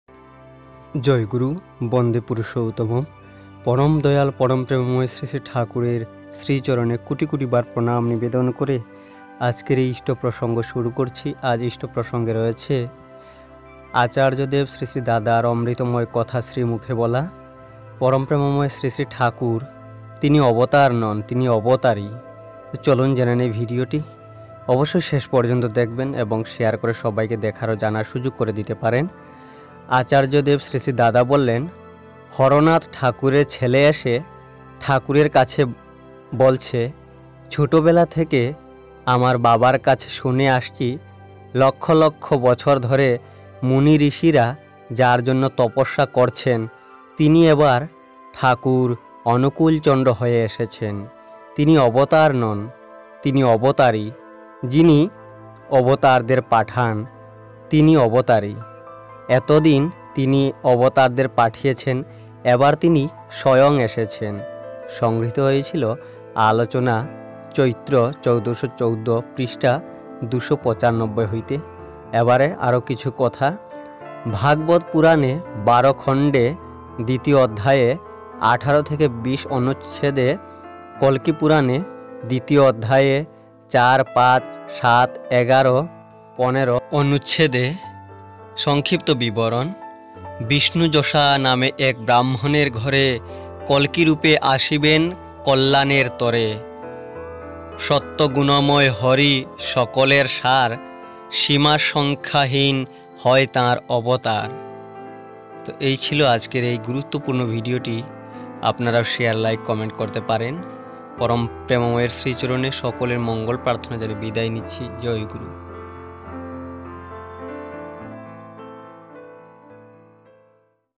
শ্রীশ্রীঠাকুর অবতার নন অবতারী,শ্রীশ্রীদাদার শ্রীমুখের বলা আলোচনা,জেনে নিন,Srisri Thakur,Srisri Dada
🙏Joyguru🙏